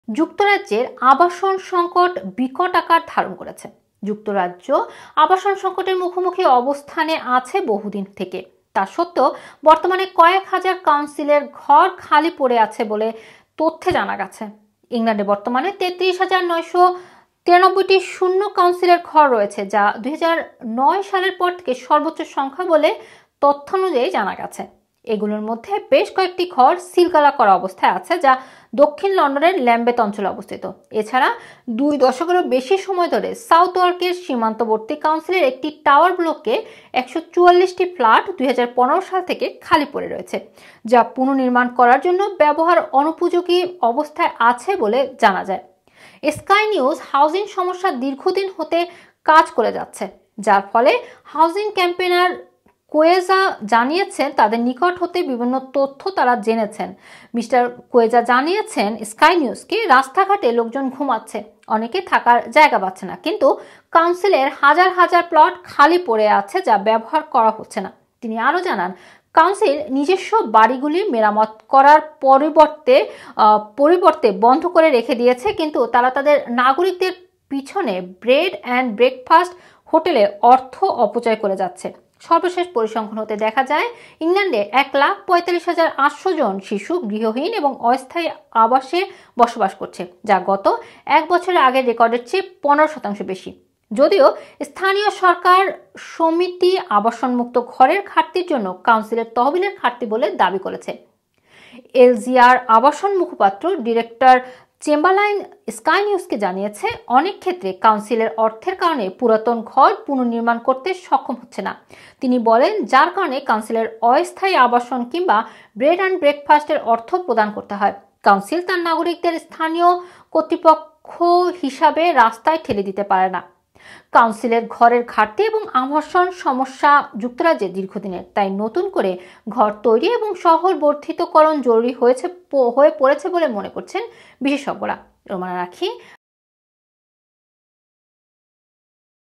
থাকার জায়গা নেই ! কিন্তু হাজার হাজার কাউন্সিলের ঘর খালি পড়ে আছে ! Runner Tv | Rumana Rakhi
থাকার জায়গা নেই ! কিন্তু হাজার হাজার কাউন্সিলের ঘর খালি পড়ে আছে !